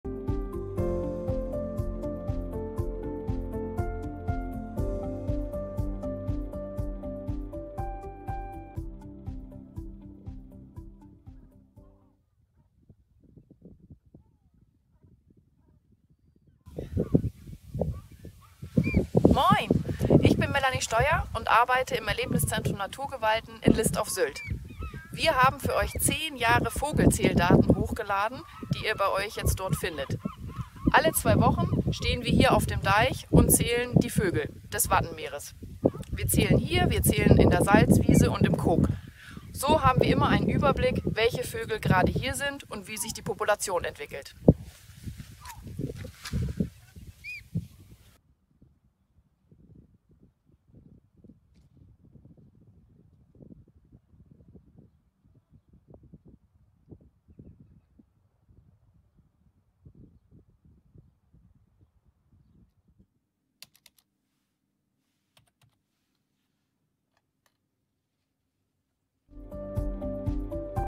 CdV SH 2021: Erlebniszentrum Naturgewalten Sylt | Vogelzähldaten des Erlebniszentrums
Coding da Vinci Schleswig-Holstein 2021 – Präsentation des Datensets "Vogelzähldaten des Erlebniszentrums" / Erlebniszentrum Naturgewalten Sylt